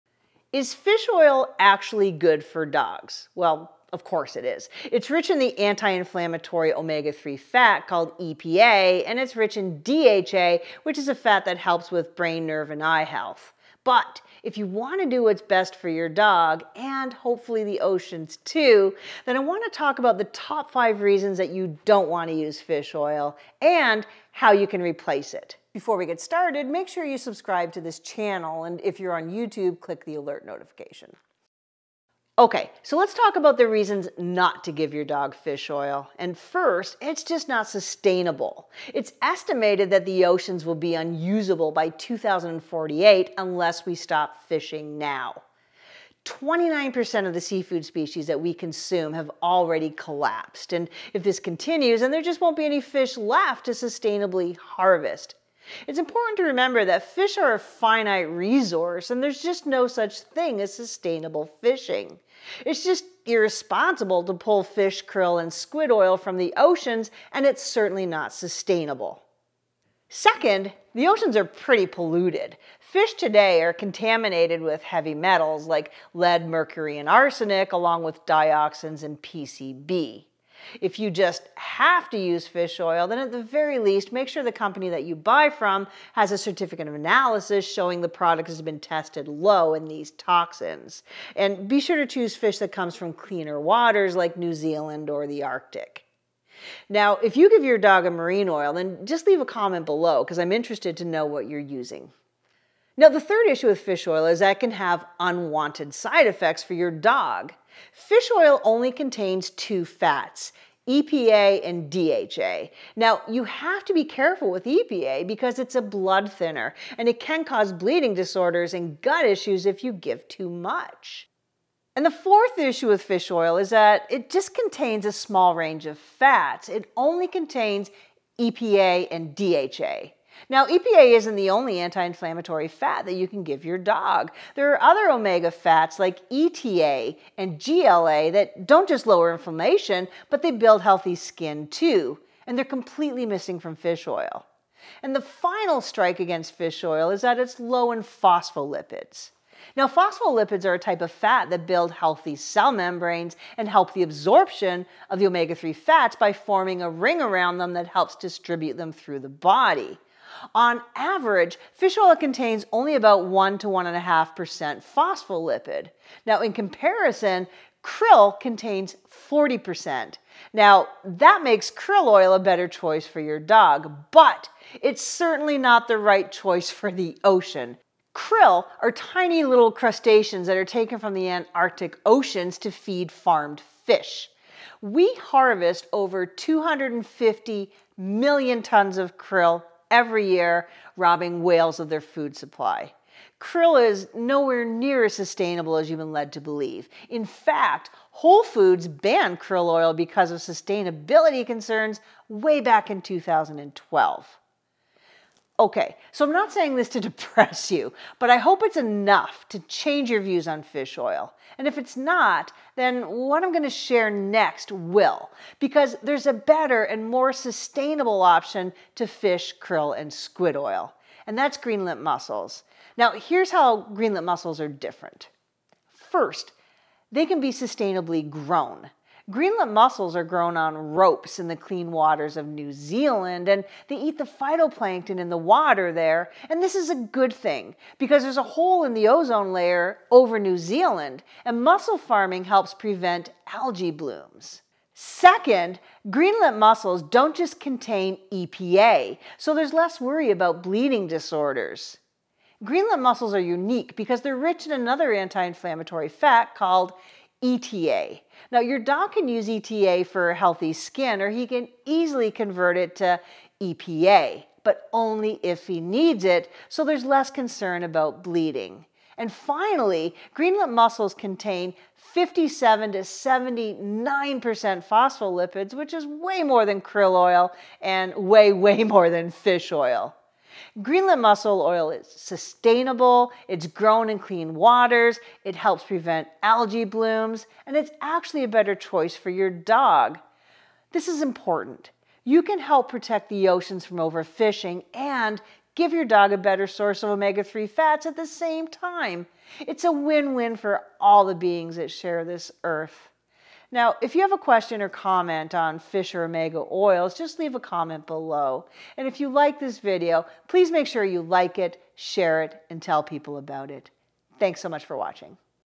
Fish Oil For Dogs: Good Or Bad?
📌 Want to learn how to make your own raw or cooked food and treats?

Is fish oil good for dogs? Well, of course it is! It's rich in the anti-inflammatory omega-3 fat EPA and it's rich in DHA, which is a fat that helps with brain, nerve and eye health. But if you want to do what's best for you dog - and hopefully the oceans too - then I want to talk about the top 5 reasons you don't want to use fish oil … and how you can replace it.

OK so let's talk about the reasons not to give your dog fish oil. And first, it's just not sustainable. It's estimated that the oceans will be unusable by 2048 unless we stop fishing now. 29% of the seafood species we consume have already collapsed and, if this continues, there won't be any fish left to sustainably harvest. It's important to remember that fish are a finite resource and there's just no such thing as sustainable fishing. It's just irresponsible to pull fish, krill and calamari from the oceans and it's certainly not sustainable. Second, the oceans are pretty polluted. Fish today are contaminated with heavy metals like lead, mercury and arsenic, along with dioxins and PCB. If you just have to use fish oil, then at least make sure the company you buy from has a certificate of analysis or COA showing the product has been tested low in these toxins. And choose fish that come from cleaner waters like New Zealand or the arctic.

The third issue with fish oil is that it can have unwanted side effects for your dog. Fish oil only contains mostly EPA. Now, you have to be careful with EPA because it's a blood thinner and can cause bleeding disorders and gut issues if you give too much.

And the fourth issue with fish oil is, again, that it only contains a small range of fats. EPA isn't the only anti-inflammatory fat you can give your dog - there are other omega fats like EPA and GLA that don't just lower inflammation, but build healthy skin too - and they're completely missing from fish oil.

And the final strike against fish oil is that it's low in phospholipids. Now, phospholipids are a type of fat that build health cell membranes and help with the absorption of omega-3 fats by forming a ring around them that helps distribute them through the body. On average, fish oil contains only 1 - 1.5% phospholipid. In comparison, krill contains 40%. Now, that makes krill oil a better choice for your dog - but it's certainly not the right choice for the ocean. Krill are tiny crustaceans that are taken from Antarctica oceans to feed farmed fish. We harvest over 250 million tons of krill every year, robbing whales of their food supply. Krill is nowhere as sustainable as you've been led to believe -  in fact, Whole Foods banned krill oil because of sustainability concerns way back in 2012.

OK so I'm not saying this to depress you … but I hope it's enough to change your views on fish oil. And if it's not, then what I'm going to share next will - because there's a better and more sustainable options to fish, krill and squid oil. And that's green lipped mussels. Here's how GLM are different:

First, they can be sustainable grown. GLM are grown on ropes in the clean waters of New Zealand and they eat the phytoplankton in the water. And this is a good thing, because there's a hole in the ozone layer over New Zealand and mussel farming helps prevent algae blooms. Second, GLM don't just contain ETA so there's less worry about bleeding disorders - GLM are unique because they're rich in another anti-inflammatory fat called ETA. Your dog can use ETA for healthy skin or he can easily convert it to EPA - but only if he needs it, so there's less concern about bleeding. And finally, GLM contains 57 - 79% phospholipids, which is a lot more than krill oil and way more than fish oil. GLM oil is sustainable, it's grown in clean waters, it helps prevent algae blooms - and it's actually a better choice for your dog. This is important - you can help protect the oceans from over-fishing and give your dog a better source of omega-3 fats at the same time. It's a win-win for all the beings that share this earth. And if you like this video, please make sure you like it, share it, and tell people about it.